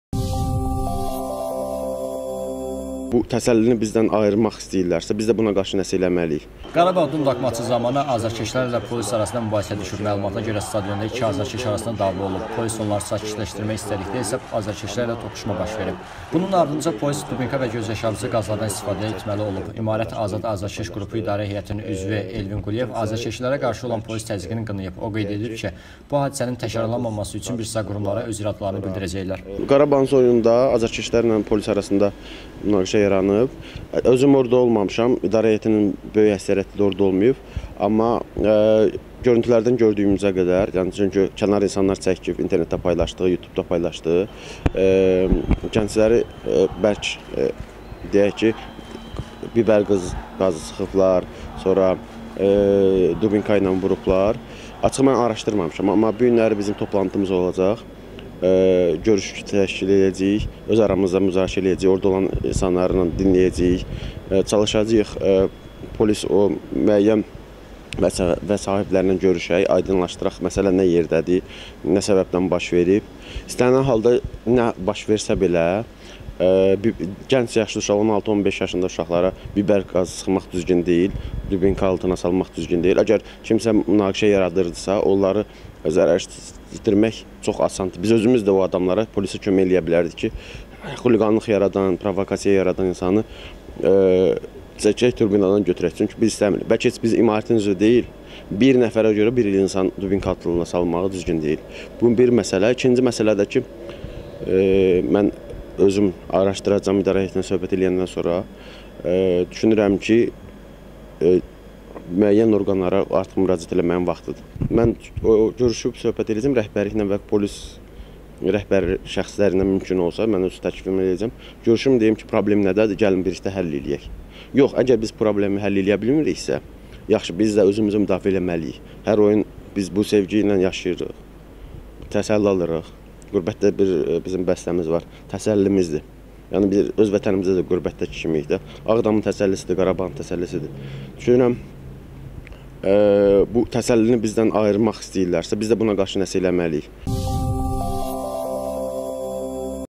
"Qarabağ"ın oyununda gənclər niyə "dubinka" altına salınıb?Elvin Quliyev şərh edir
"Qarabağ" - "Dundalk" matçı zamanı azarkeşlərlə polis arasında mübahisə düşüb. Məlumata görə, stadionda iki azarkeş arasında dava olub. Polis onları sakitləşdirmək istədikdə isə azarkeşlərlə toqquşma baş verib. Bunun ardınca polis "dubinka" və gözyaşardıcı qazlardan istifadə etməli olub. "İmarət" azad azərkeş qrupu idarə heyətinin üzvü Elvin Quliyev azərkeşlərə qarşı olan polis təzyiqini qınayıb. O, qeyd edib ki, bu hadisənin təkrarlanmaması üçün bir sıra qurumlara öz iradlarını bildirəcəklər.